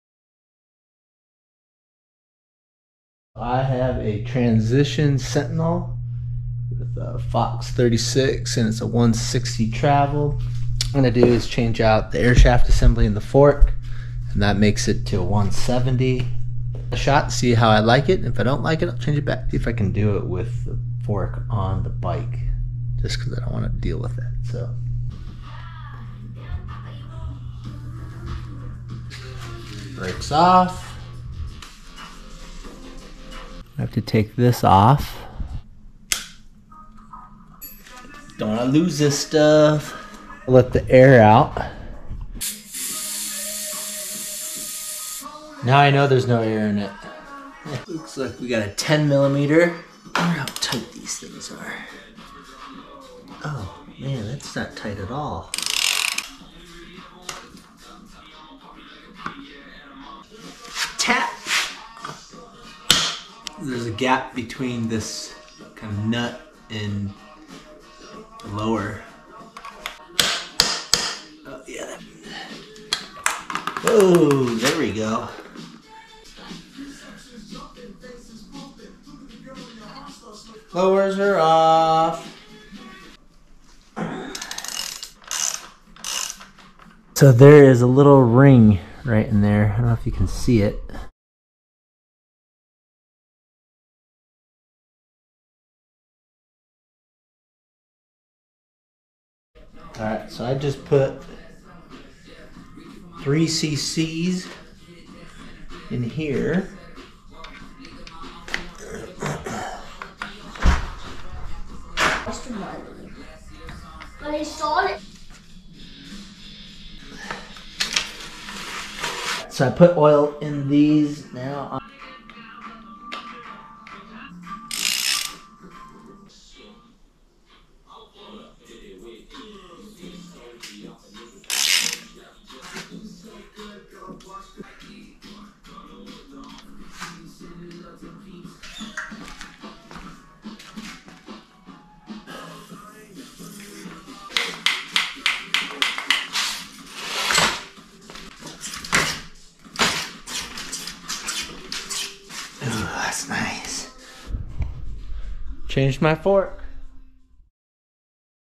Changing the air shaft assembly in a Fox Float 36 fork.
The fork is a 160mm travel and converts to a 170 after changing the air shaft assembly.

The job took me about an hour. Could have been a little faster but I end up doing a lot of messing around and looking up oil compatibility and use of student oil weights.